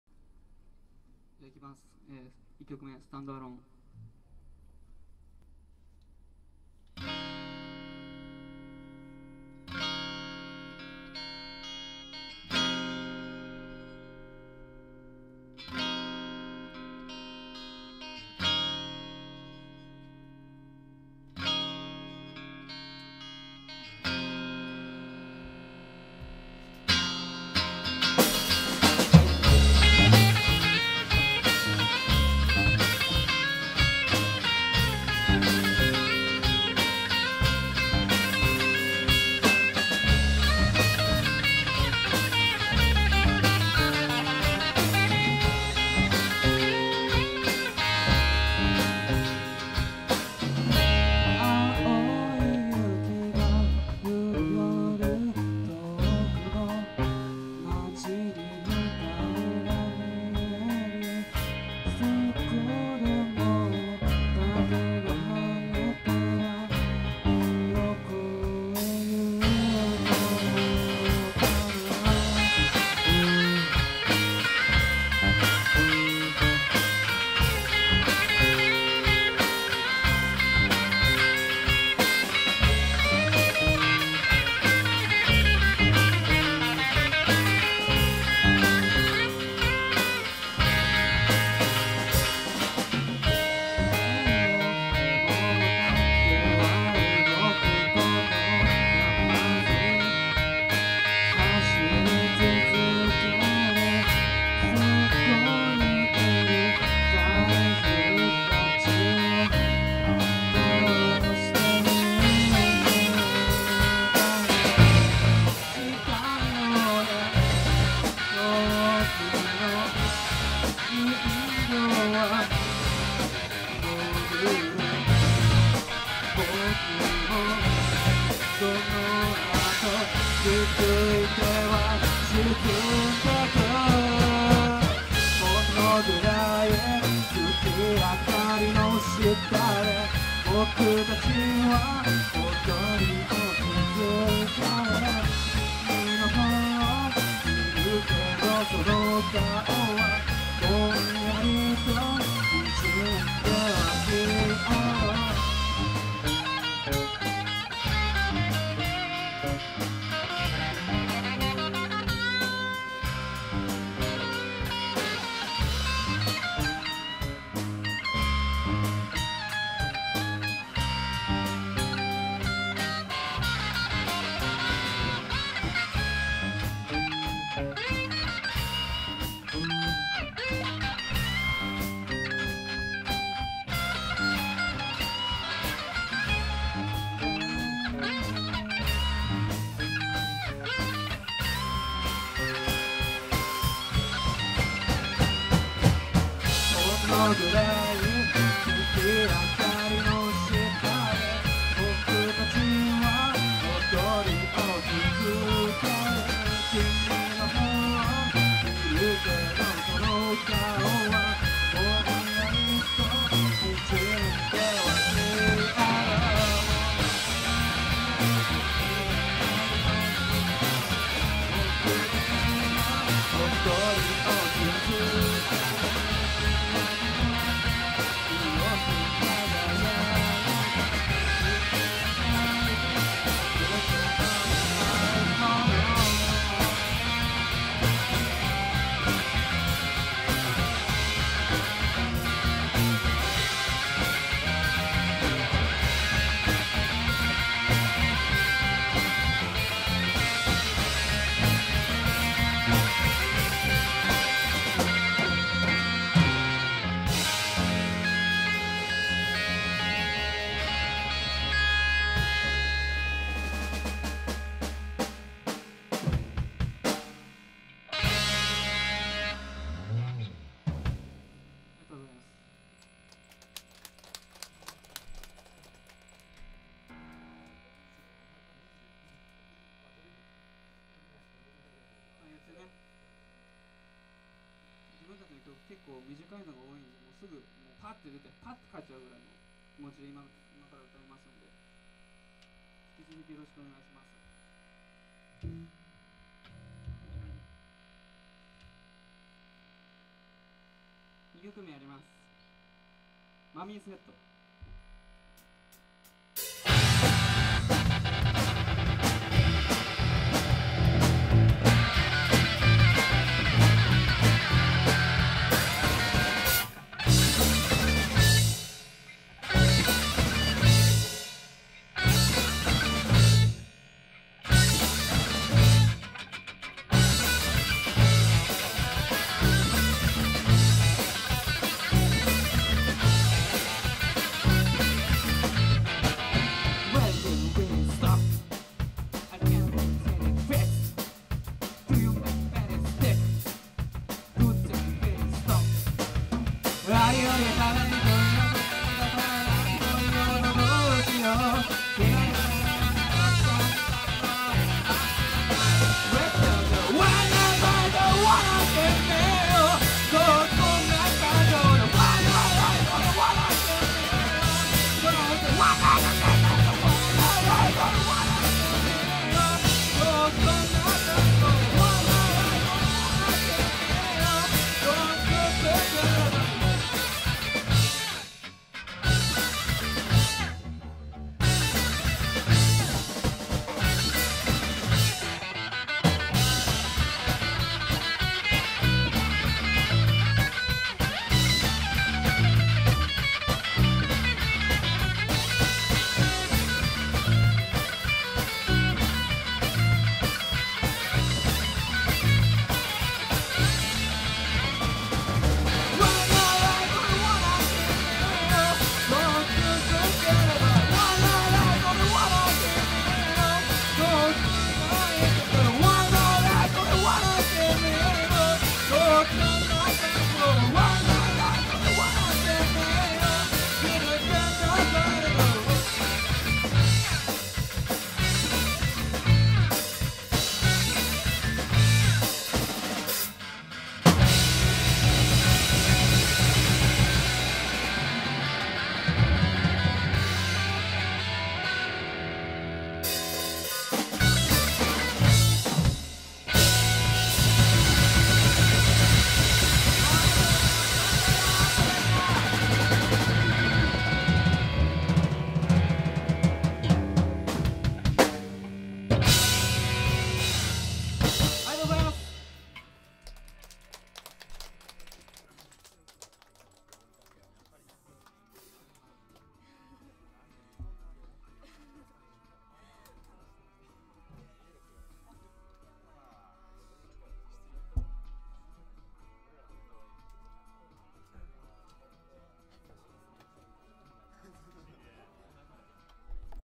Stand alone ant HOTLINE2013 島村楽器大分フォーラス店 店予選動画
島村楽器大分フォーラス店で7月21日に開催された、HOTLINE2013 店予選のレポート動画です。